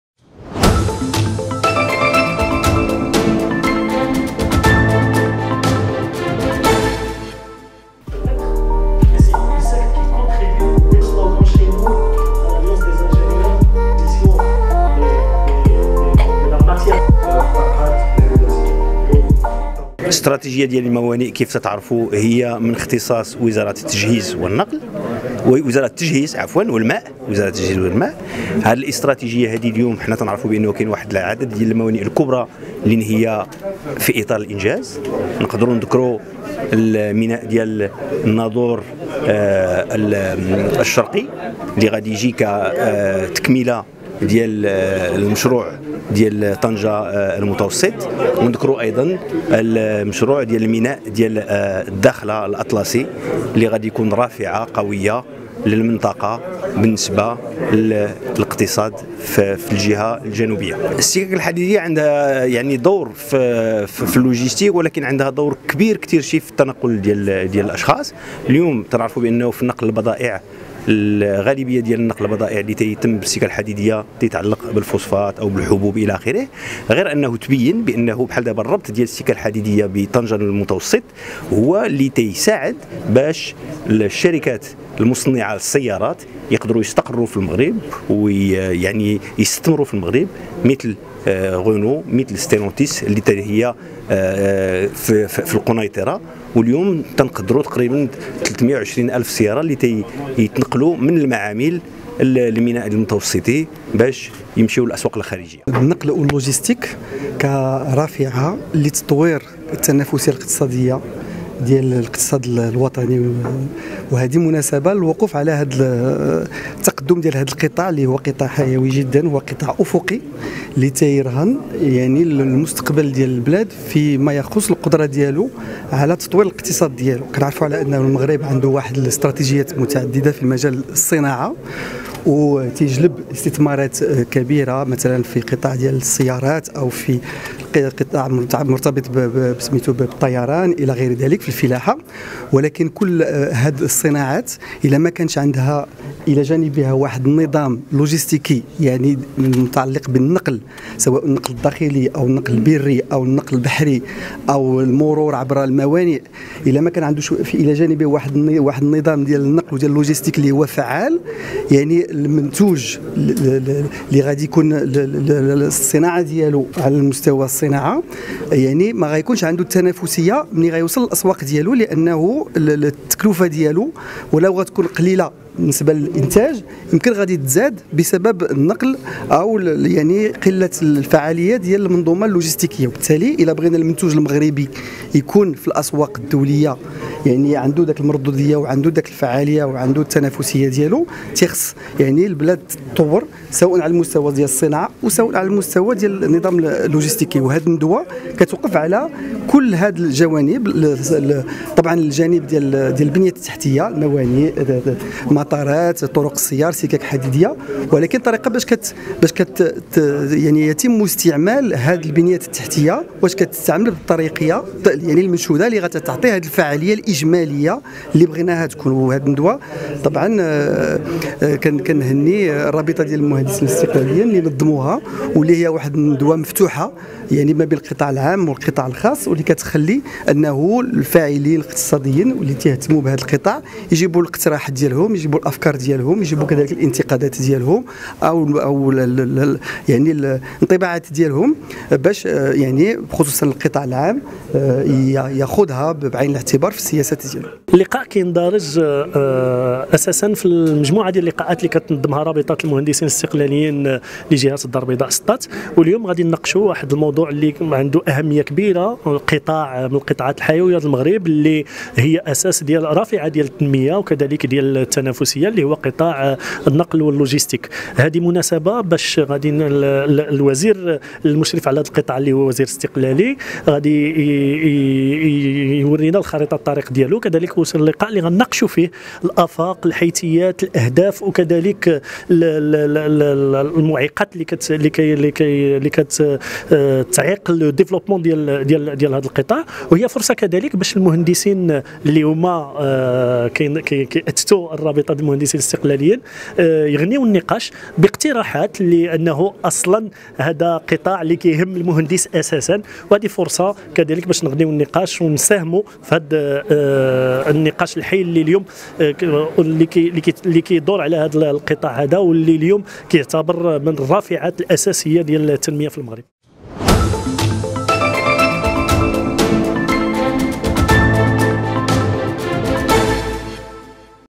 البيضاء.. تنافسية النقل واللوجستيك تحت مجهر رابطة المهندسين الاستقلاليين
نظم المكتب الجهوي الدار البيضاء سطات لرابطة المهندسين الاستقلاليين ندوة علمية حول "النقل و اللوجيستيك: رافعة للتنافسية و التطور" يوم الأربعاء 26 أبريل 2023، وذلك بمشاركة محمد عبد الجليل، وزير النقل و اللوجيستيك، وكريم غلاب، وزير سابق إلى جانب أعضاء من رابطة المهندسين وأطر حزب الاستقلال.